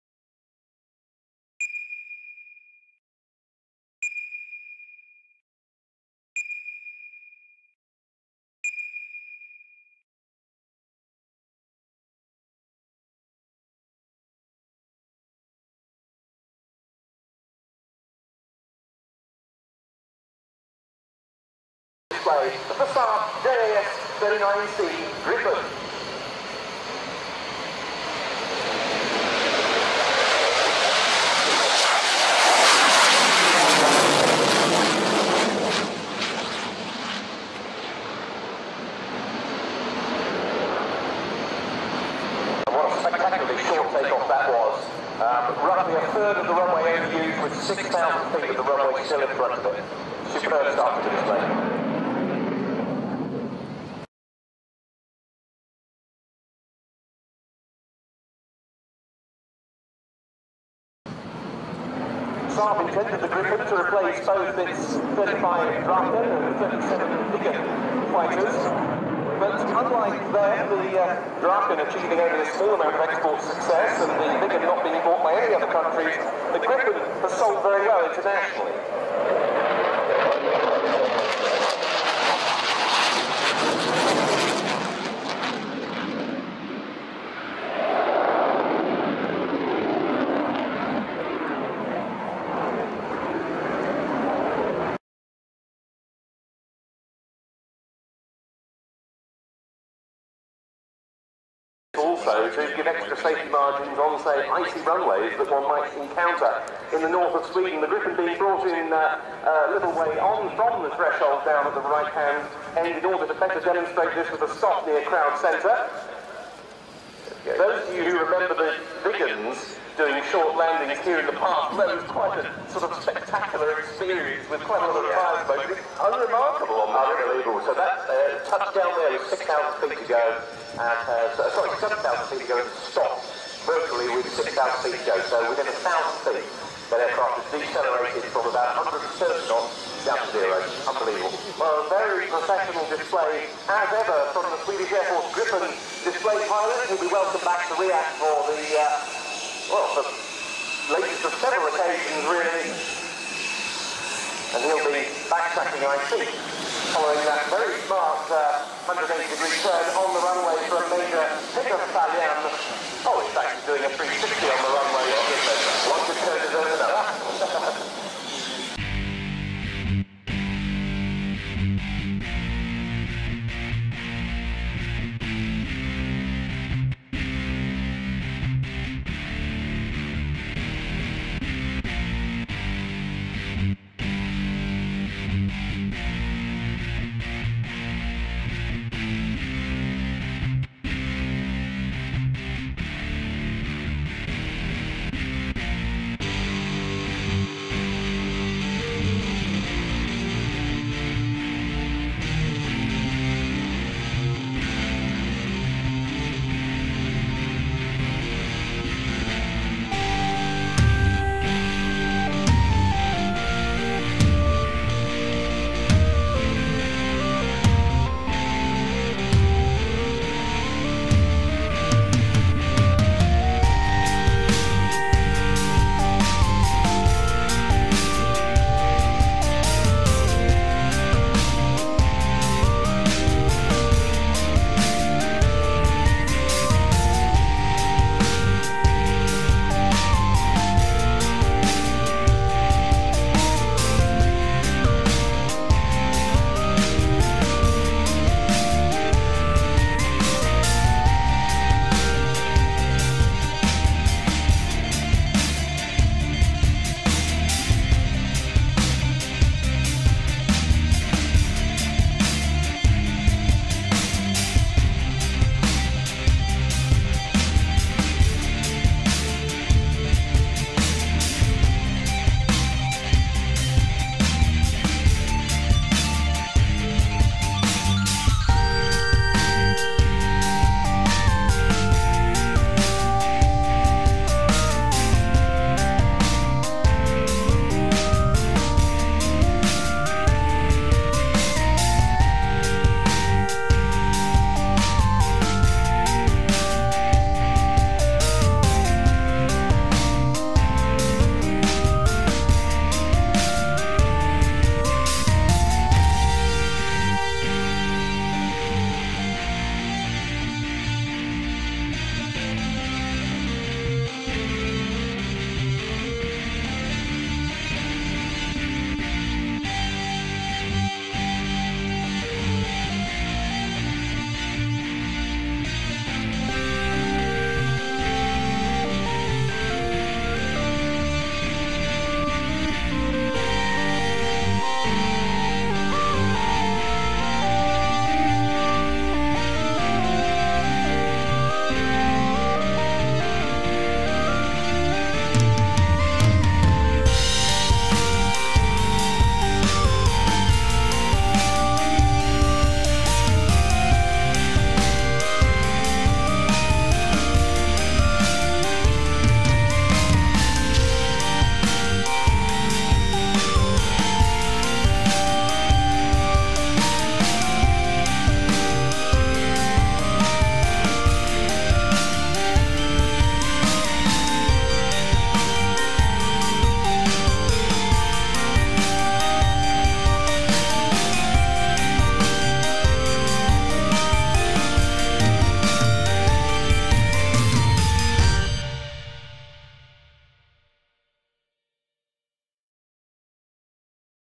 REVELL (Italeri) JAS-39 Gripen - A Build In Pictures
Thank you to Blizzardthewatcher for the use of his
excellent video

GBG logo by I.P. & R.A

RBMVideos 2022